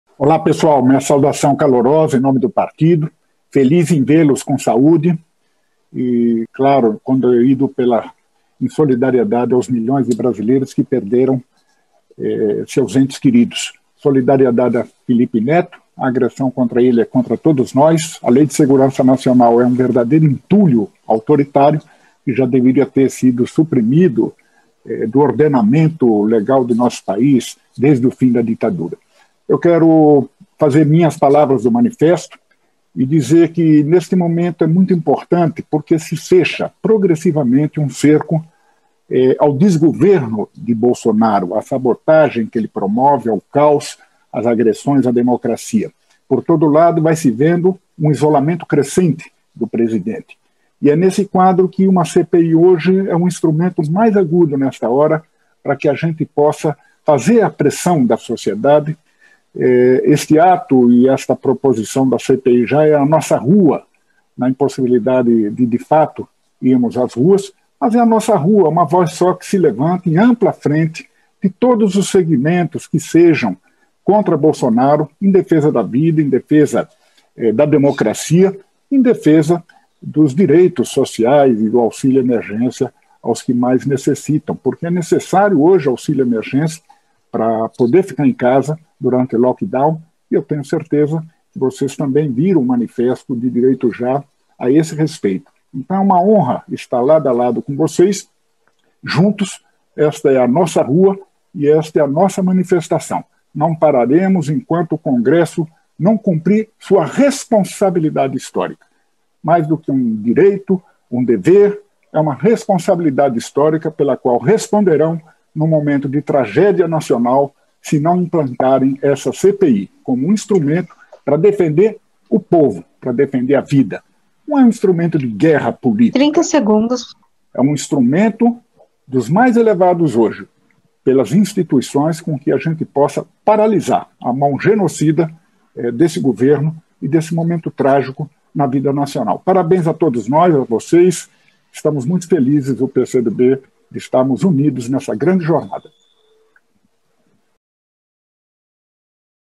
Walter Sorrentino diz que CPI da Pandemia é o instrumento mais preciso em defesa da vida
Walter Sorrentino, vice-presidente do PCdoB, participa do VII Ato do Direitos Já! em solidariedade a Felipe Neto e pela CPI da Pandemia. A CPI é o instrumento mais preciso agora em defesa da vida, disse Sorrentino